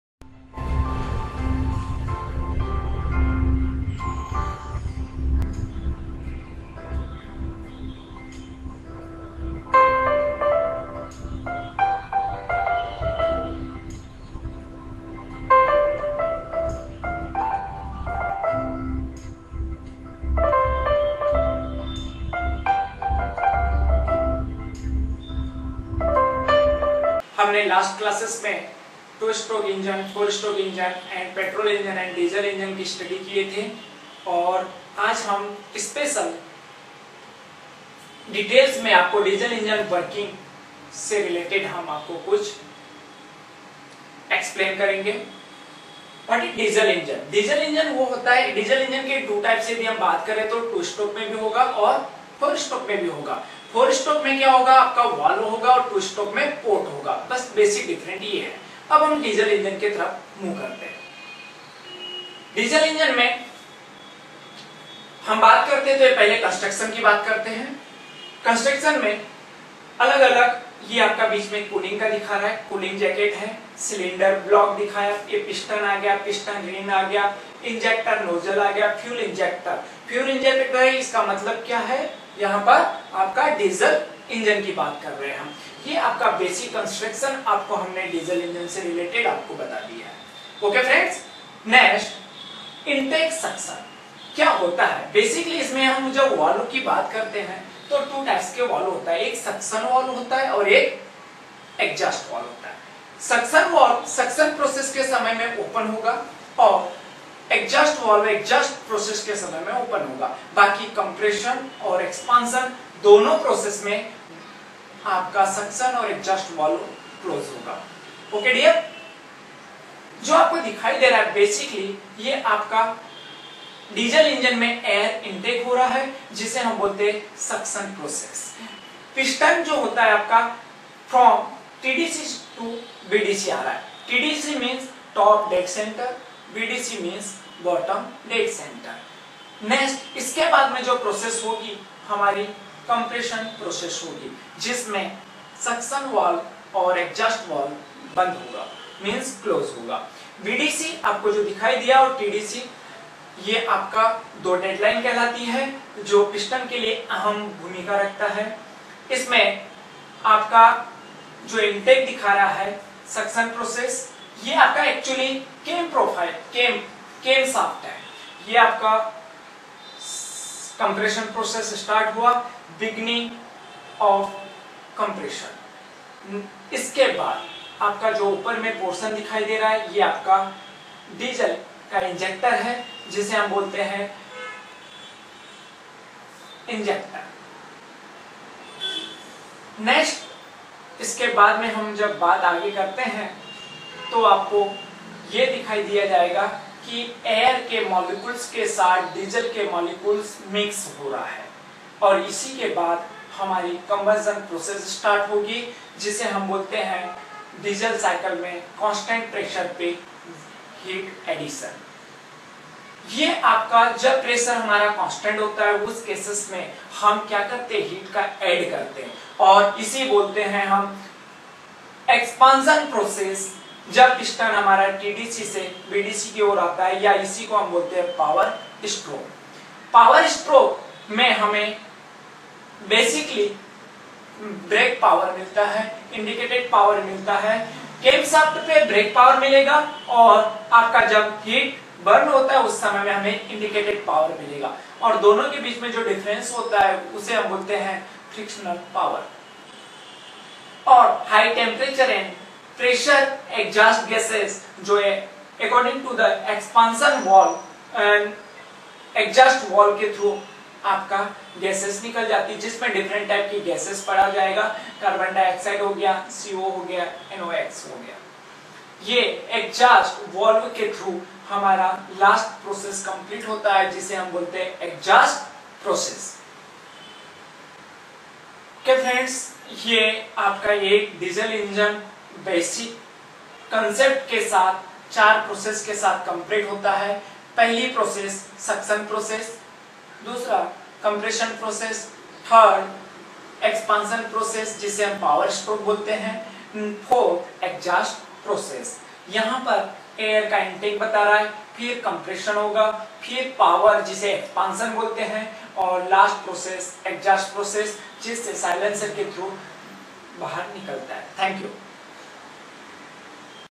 08A2016AAA-Working Process of Diesel Engine by Amar Armour in Hindi & English
described about diesel Engine and its parts  and their different- different  work and four types  process